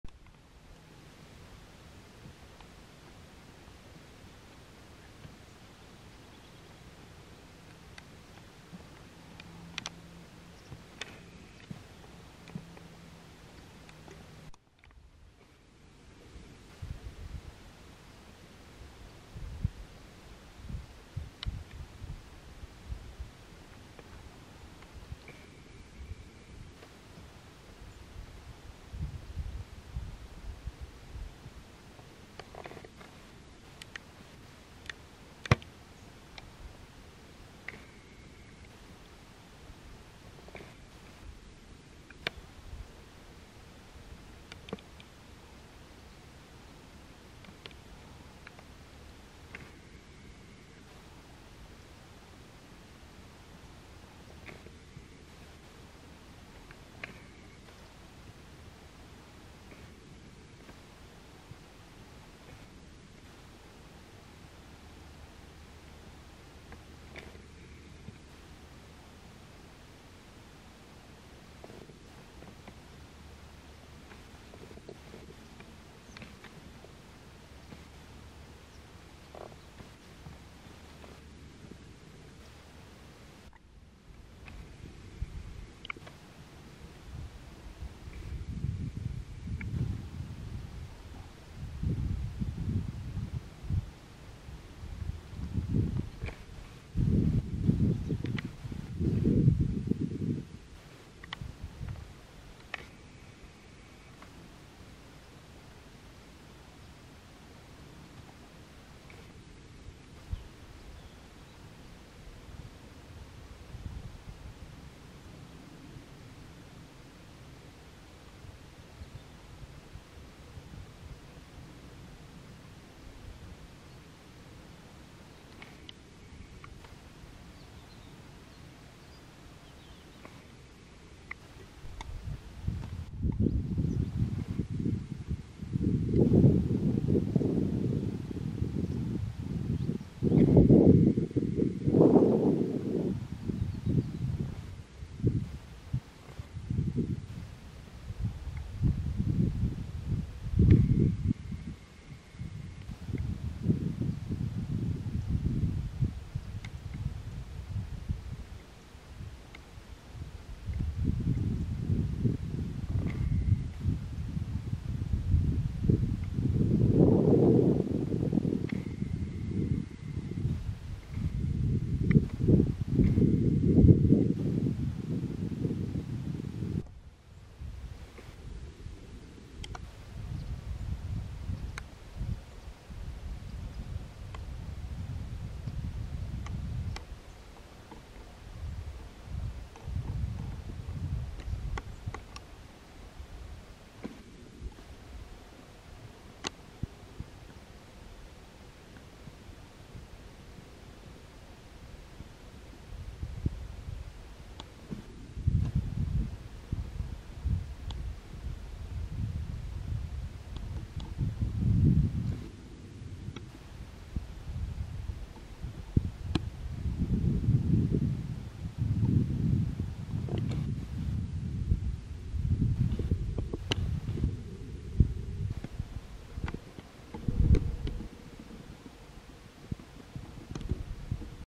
Cavalos Sorraia (Equus ferus caballus) na Quinta do Pisão
Esta é uma raça portuguesa em perigo de extinção que teve origem na zona de confluência entre as ribeiras de Sor e da Raia (daí o seu nome), charneca de Coruche, onde haveria uma extensa população, popular entre criadores de gado para trabalhos do campo. Filmados na Quinta do Pisão, Cascais.